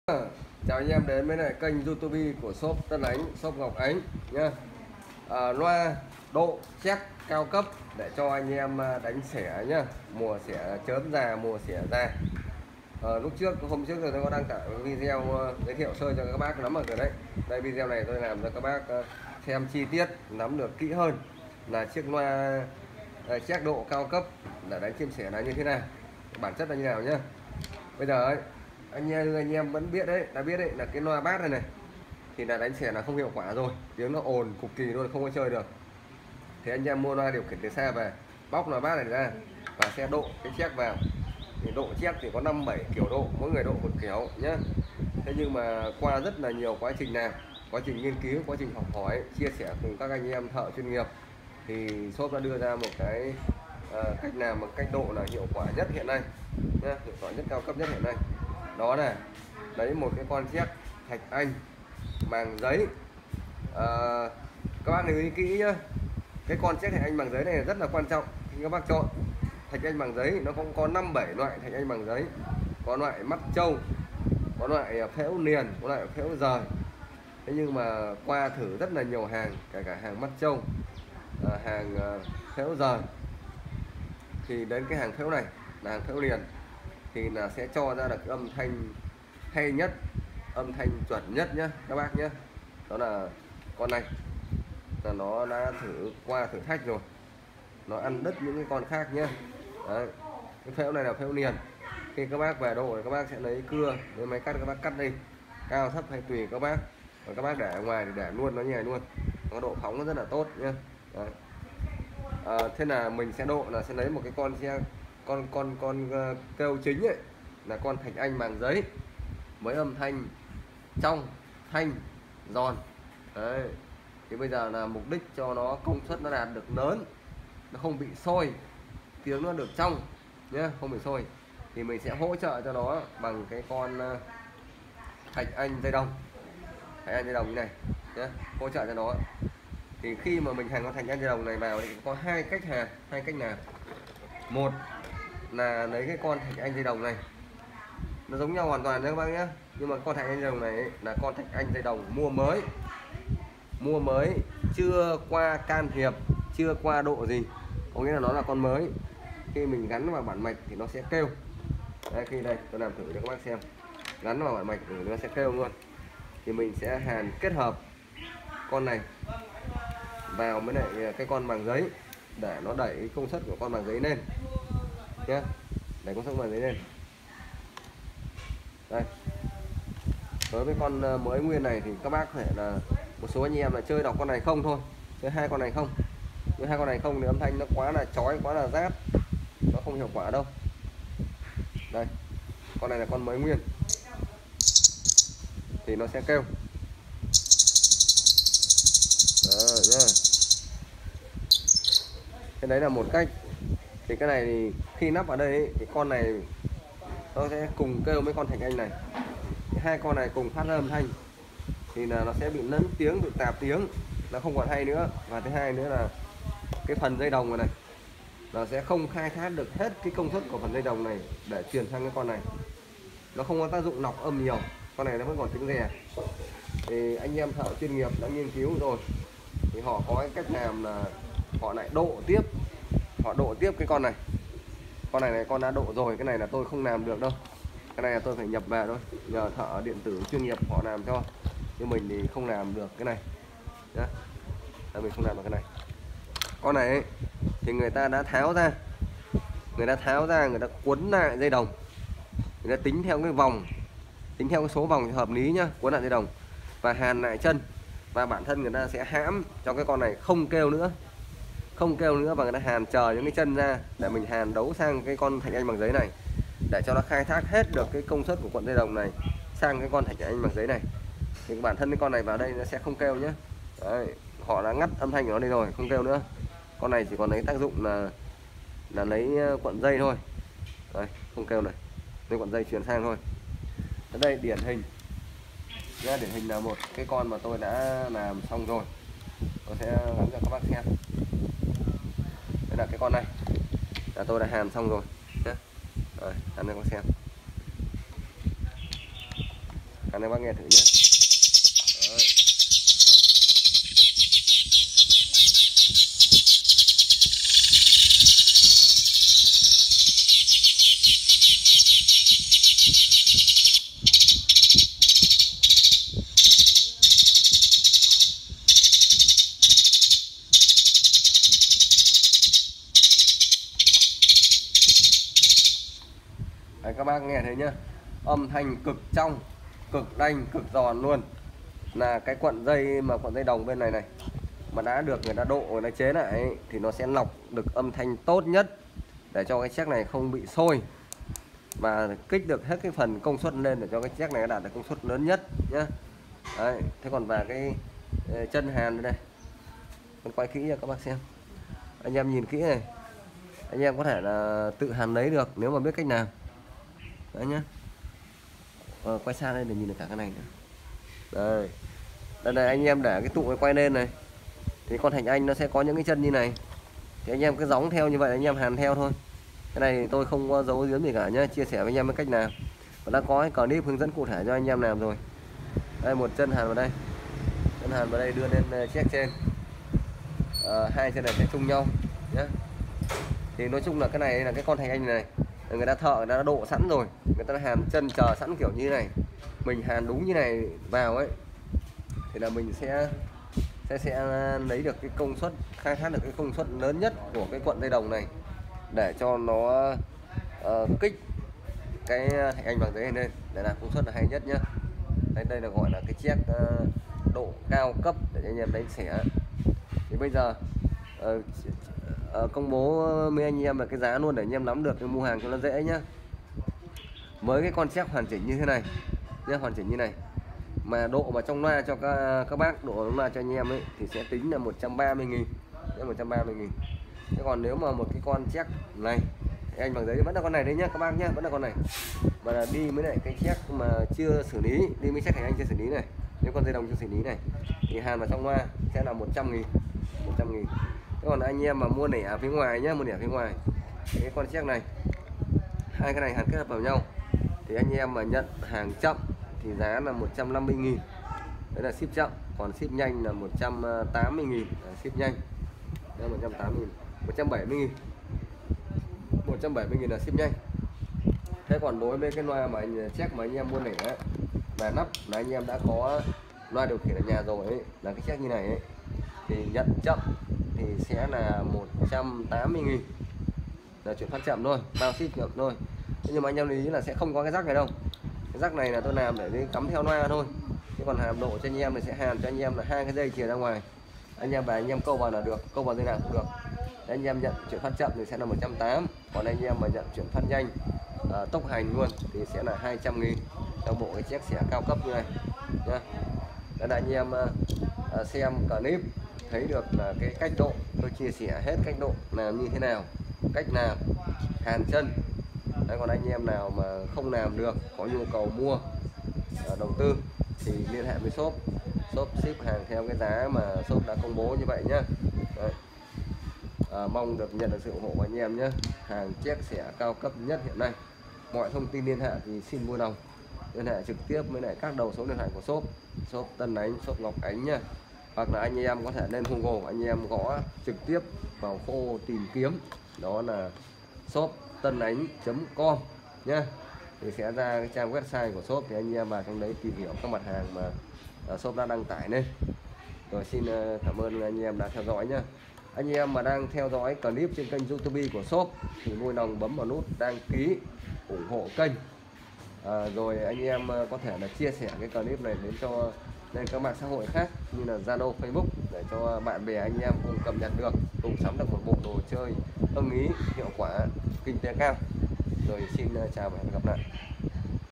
LOA ĐIỀU KHIỂN ĐỘ SIÊU CHÉC CHUYÊN BẪY CHIM SẺ GIÀ, SẺ CHỚM GIÁ HIỆU QUẢ 0393423848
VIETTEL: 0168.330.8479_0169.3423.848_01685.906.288
LOA ĐIỀU KHIỂN TỪ XA ĐỘ SIÊU CHÉC CHO ANH EM  THỢ BẪY CHUYÊN NGHIỆP. ÂM THANH CHUẨN PHÙ HỢP BẪY SẺ GIÀ VÀ SẺ CHỚM GIÀ HIỆU QUẢ CAO.